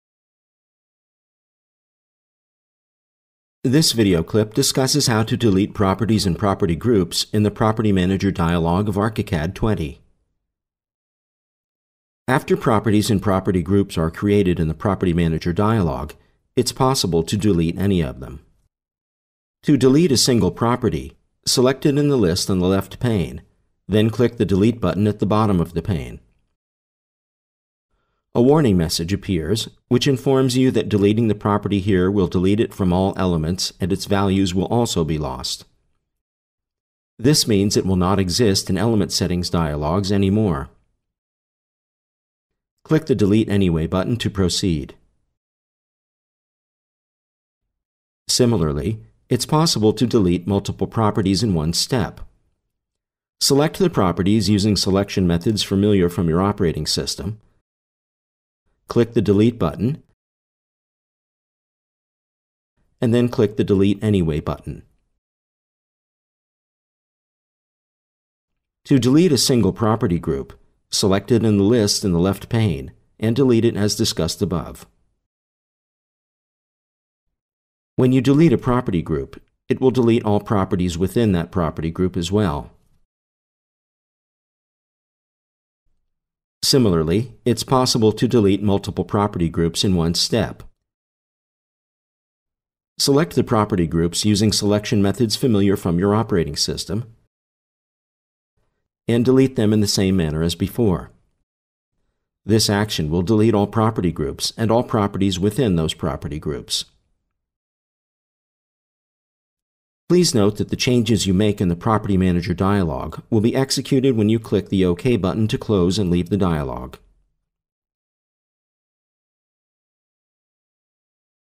Deleting Properties and Property Groups
This video clip discusses how to delete Properties and Property Groups in the Property Manager Dialog of ARCHICAD 20. After Properties and Property Groups are created in the Property Manager Dialog it is possible to delete any of them.